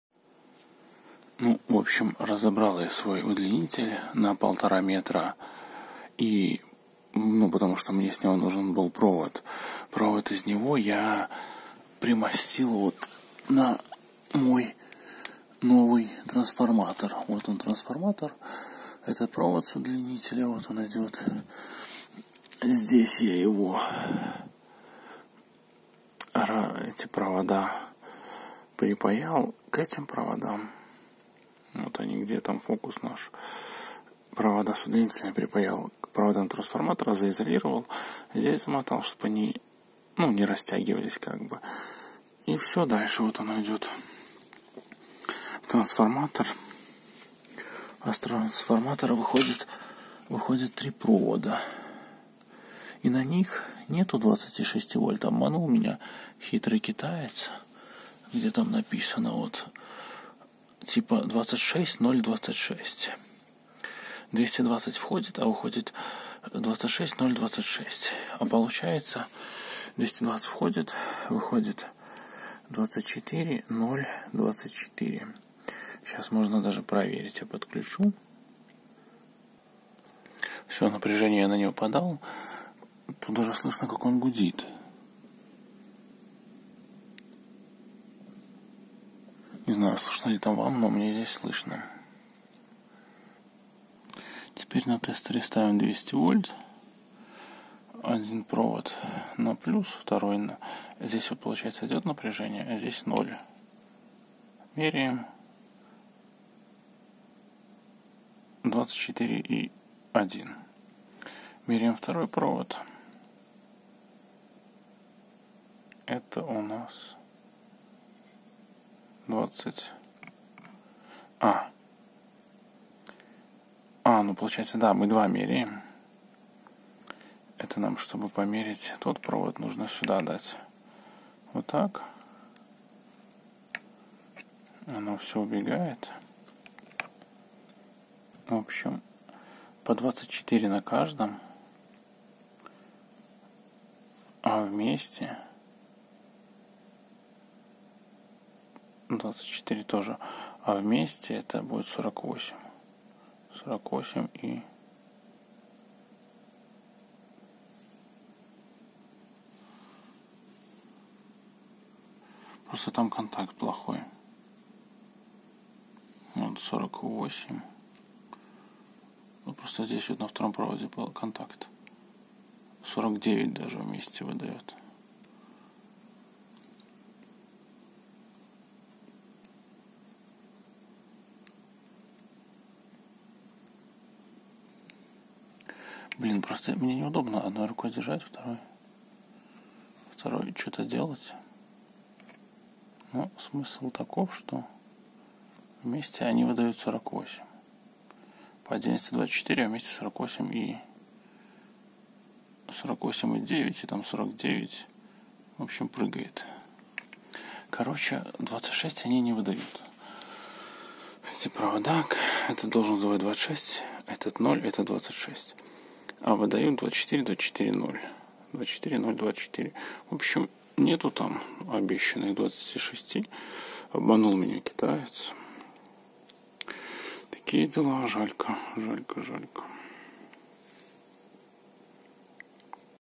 Тороидальный трансформатор 350W dual AC - припаял штепсель, проверил мультиметром
Вообщем теперь мой трансформатор имеет штепсель :) который я припаял. Правда пришлось пожертвовать своим удлинителем. Но его и удлинителем сложно назвать, там такой короткий провод был... но, тем не менее я им пользовался. Ну и вот, теперь легко можно подключать и отключать трансформатор к сети. А ещё я померил в нём напряжение и. не выдаёт он заявленные 26, а лишь только 24 вольта.

P.S. Там ещё момент такой 0:52 немного смешной :)